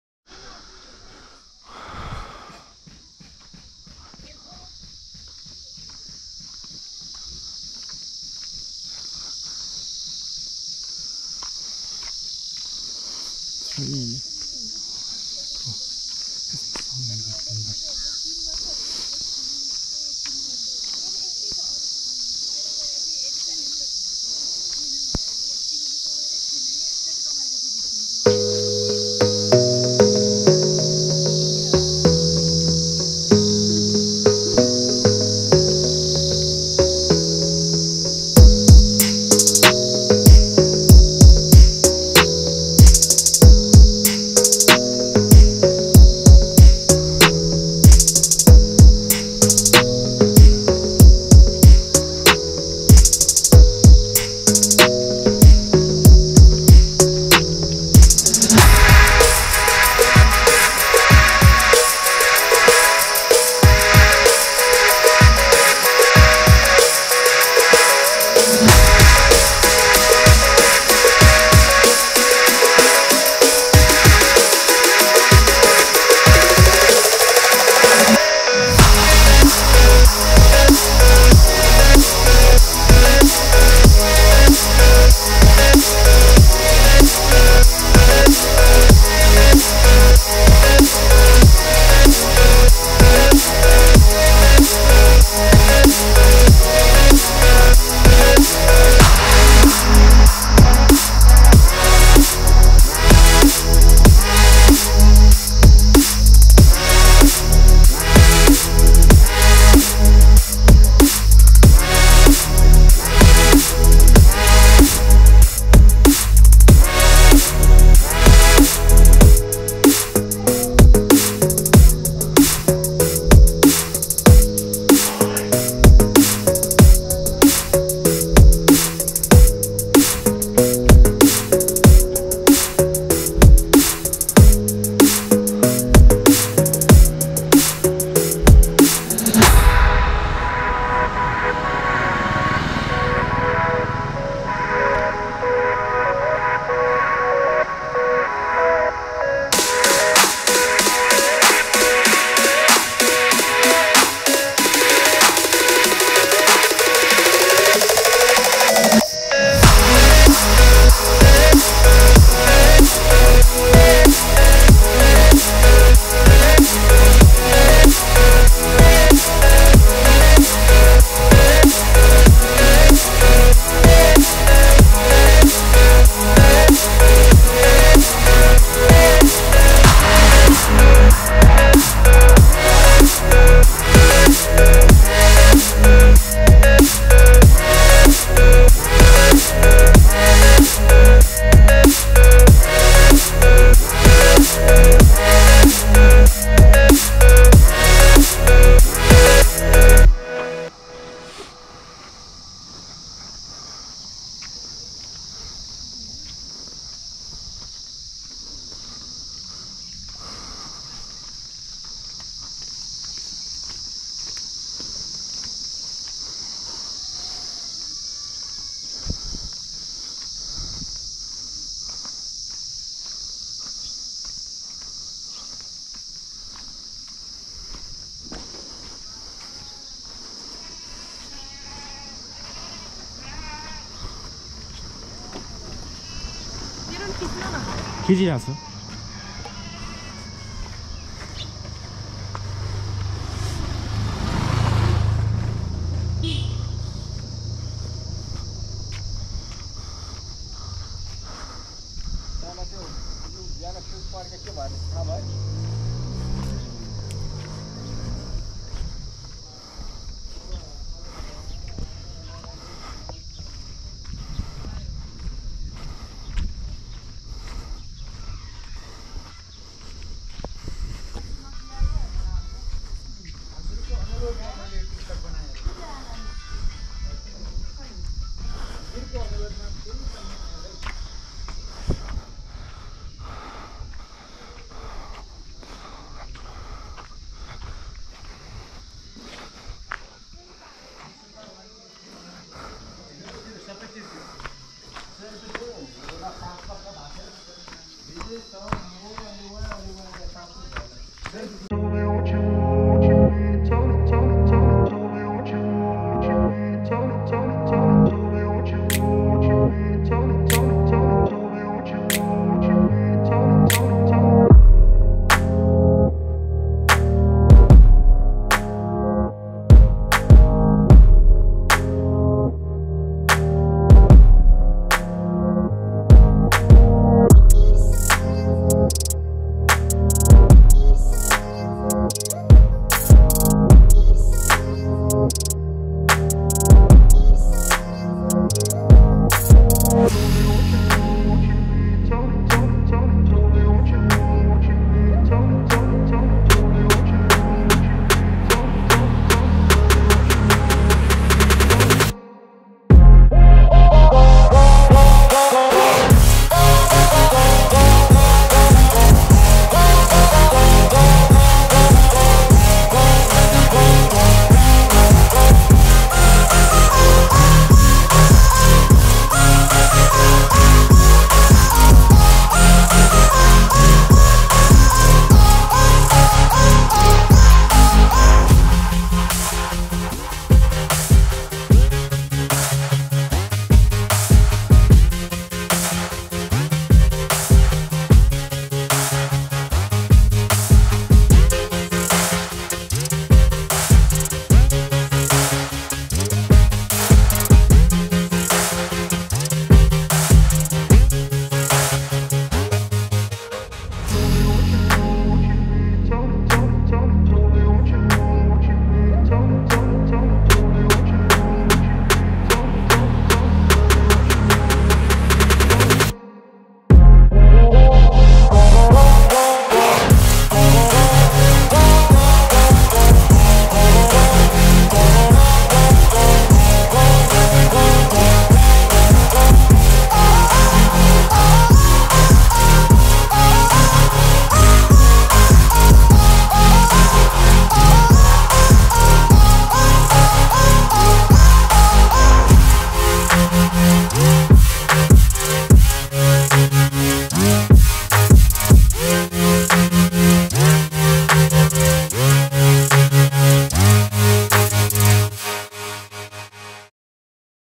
THE LOST FORGOTTEN VILLAGE | DAPCHA BAZAAR | 4K VIDEO
Dapcha Bazaar is a historical and cultural market town located in the Kavrepalanchok district of Nepal. Known for its traditional Newari architecture and serene surroundings, Dapcha offers a glimpse into Nepal's rural lifestyle. The town is situated on a hilltop, providing stunning panoramic views of the Himalayas and the surrounding valley. It is a peaceful destination for those seeking to explore the local heritage, including centuries-old temples, stone-paved streets, and authentic Newari culture.